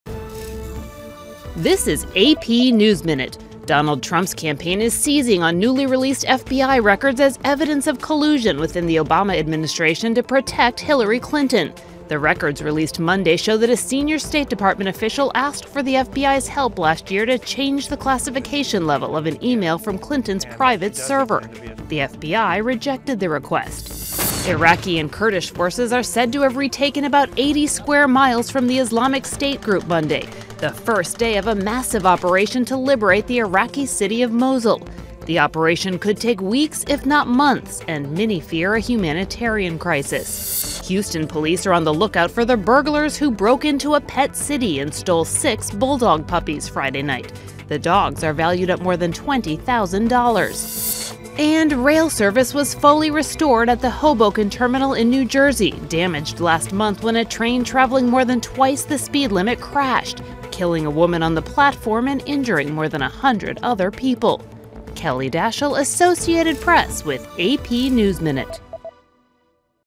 AP Top Stories
Here's the latest for Monday, October 17th: New records show State Dept. official sought to change classification of a Clinton email; Mosul offensive makes gains; Houston police look for bulldog burglars; Rail service restored in Hoboken.